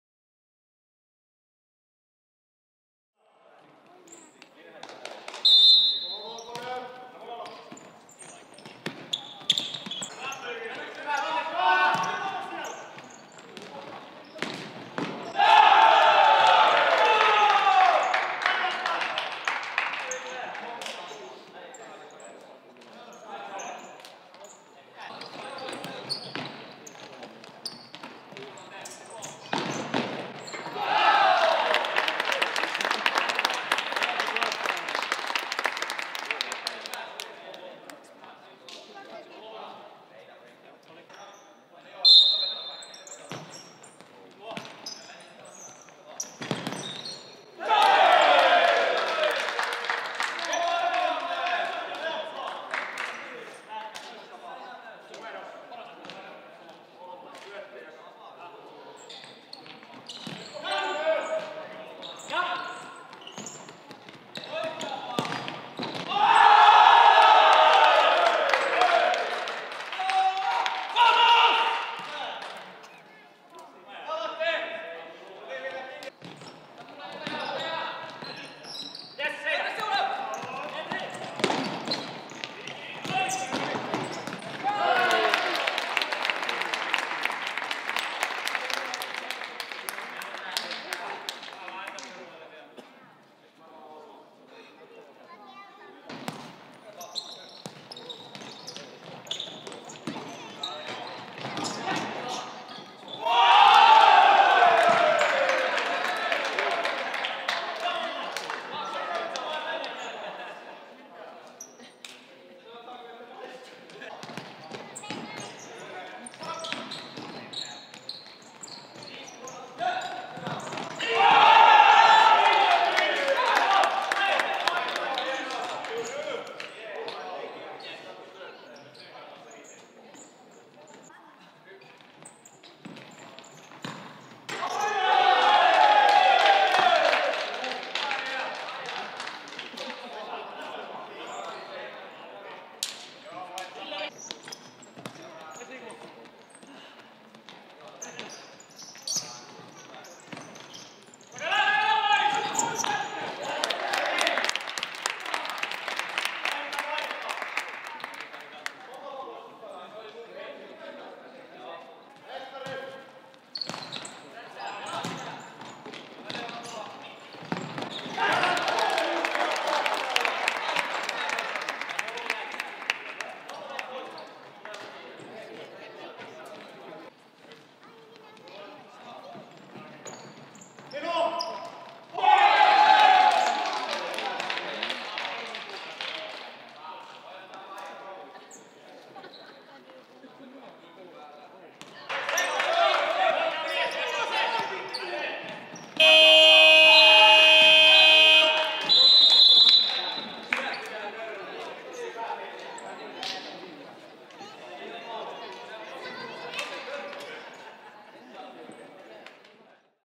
FS Feeniks - KaDy II 18.01.2020 MAALIKOOSTE!
FS Feeniks - KaDy II 7-4 (4-2)
Futsal-Kakkonen Itä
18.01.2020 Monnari

Maalit:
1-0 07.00 Laaksonen Ville (Martikainen Riku)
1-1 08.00 Lehto Patrik (Hänninen Eemeli)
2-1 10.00 Tuominen Pyry (Martikainen Riku)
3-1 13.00 Paltamaa Aleksi (Lehto Lauri)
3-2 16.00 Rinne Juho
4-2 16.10 Laaksonen Ville (Martikainen Riku)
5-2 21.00 Tuominen Pyry (Laaksonen Ville)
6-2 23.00 Paltamaa Aleksi (Lehto Lauri)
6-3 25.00 Tolvanen Kimmo (Rinne Juho)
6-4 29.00 Jalkanen Niko (Azimi Rashid)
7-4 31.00 Valtanen Teppo (Heikkinen Risto)

Produced by Lebbiz Productions